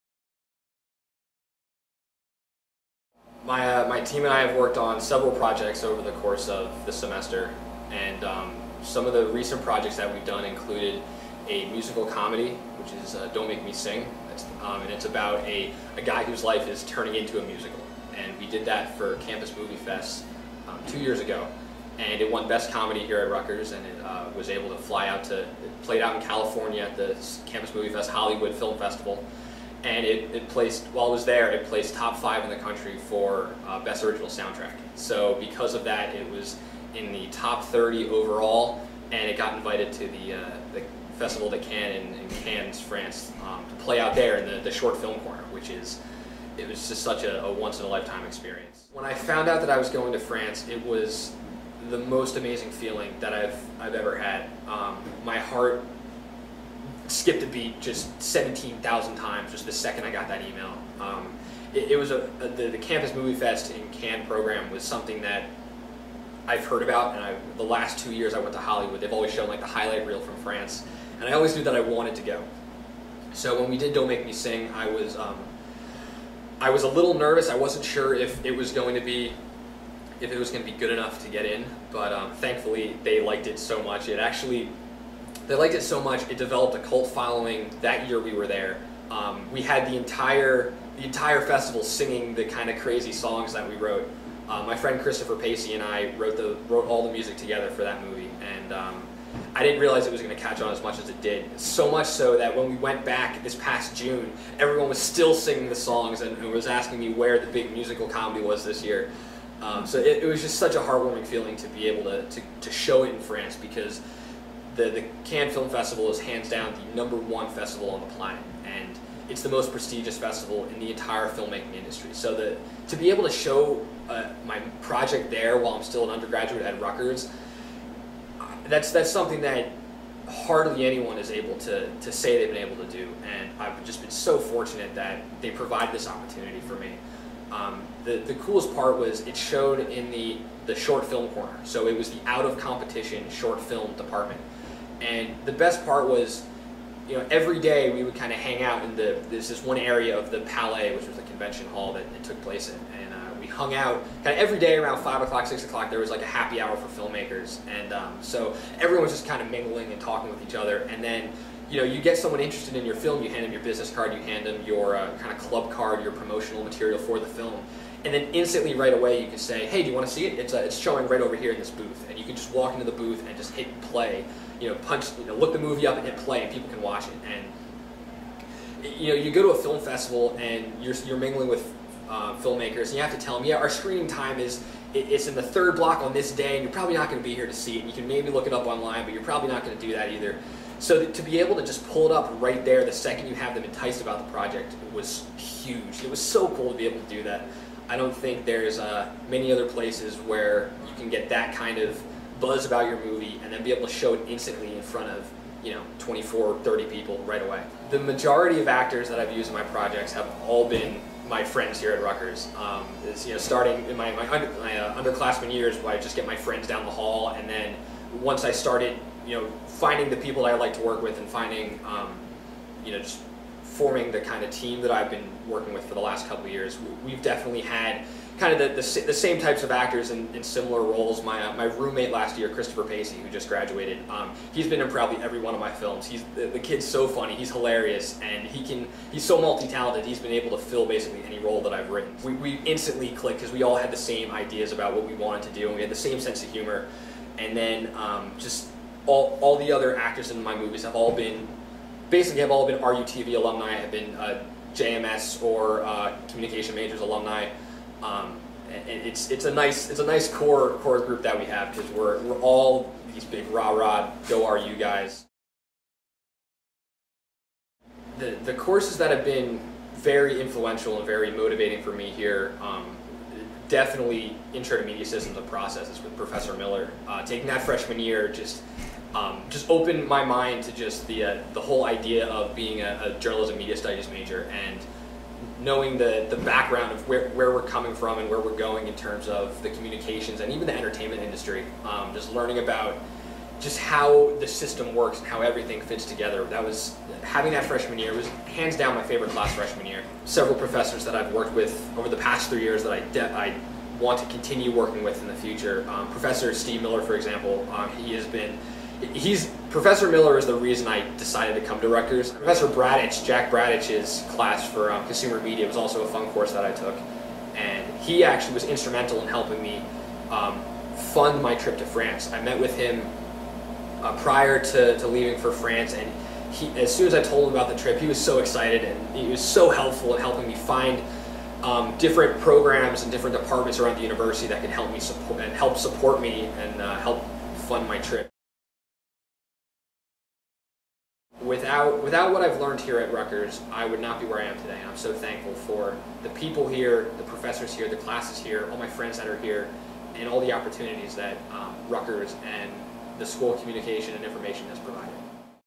JMS Senior, Zack Morrison, Shines in Cannes and Hollywood
This past summer, Zack Morrison flew to France to be there when his short film was shown at the Cannes Film Festival. He donned a tux and walked the red carpet, right behind directors Stephen Spielberg, Ang Lee, Christopher Walsh and countless other Hollywood celebrities. Not bad for a SC&I student wrapping up his junior year.